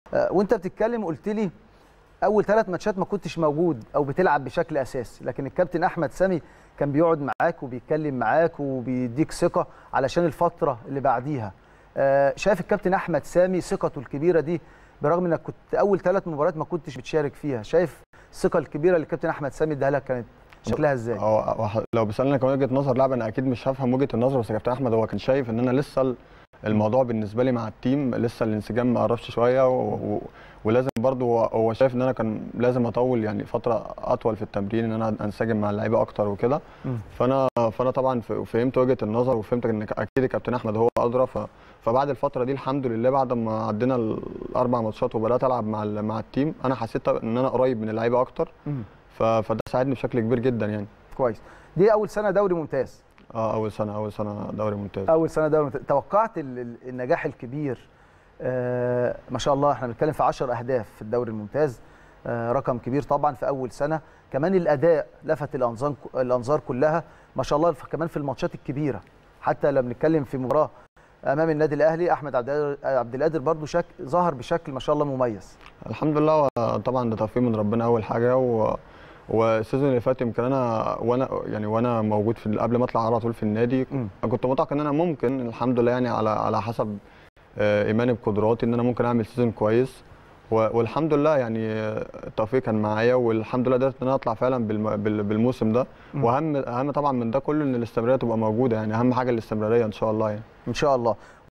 احمد عبد القادر "يكشف" أسباب تألقه مع سموحة ودور الكابتن احمد سامي معه.
احمد عبد القادر "يكشف" أسباب تألقه مع سموحة ودور الكابتن احمد سامي معه | الأشبال